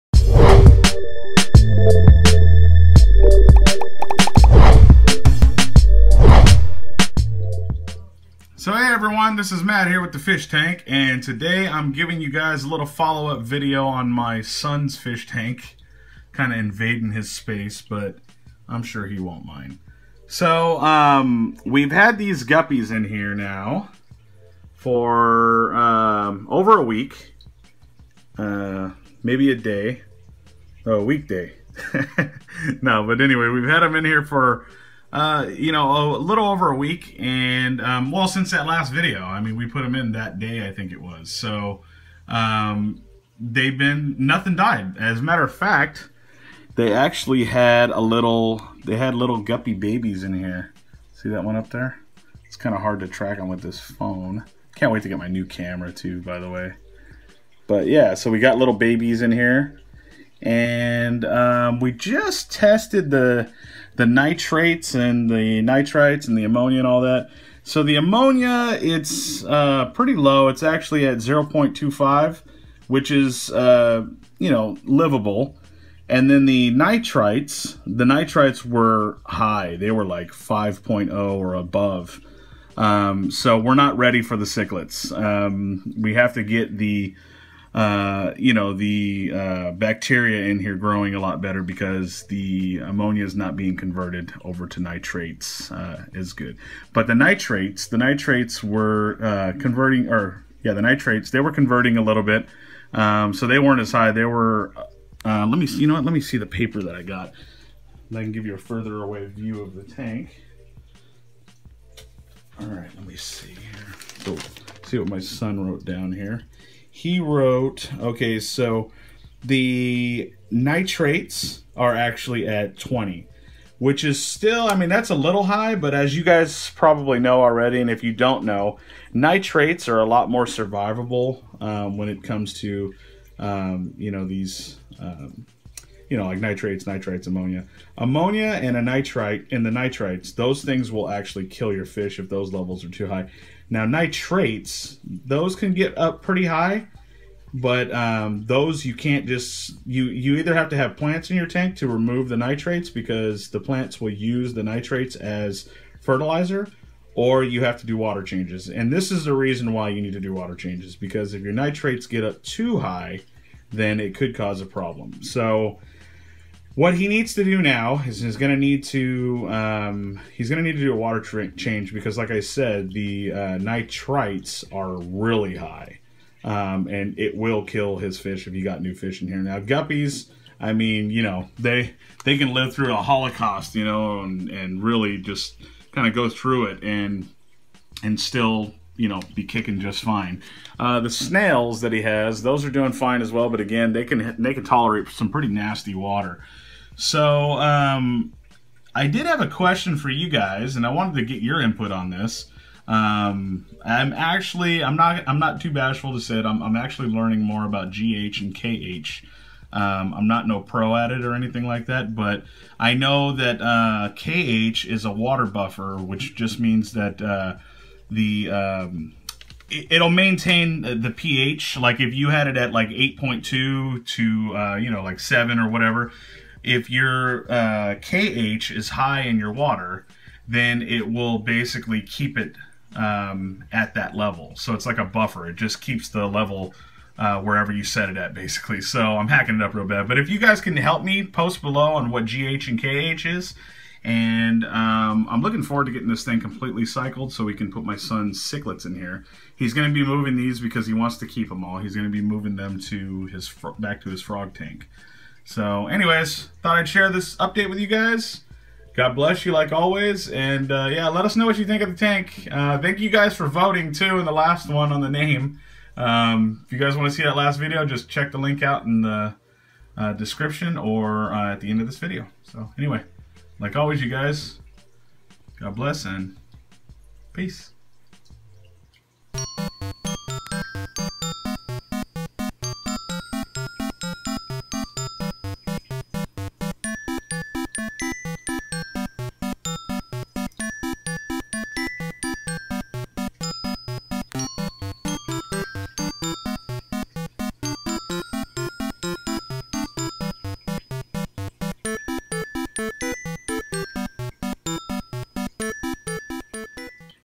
Rimless Cichlid Tank UPDATE
Well, we are still letting my sons cichlid tank cycle but the guppies are still alive and thriving so I guess things are going good:) We did test the water and this is what we got.

My numbers are as follows:
gh: 3
kh: 1
pH: 8.2
Ammonia: 0.25
Nitrite: 5.0 or above
Nitrate: 20

Nitrites are way to high to add the cichlids so we will wait to add them but I wanted to update you guys!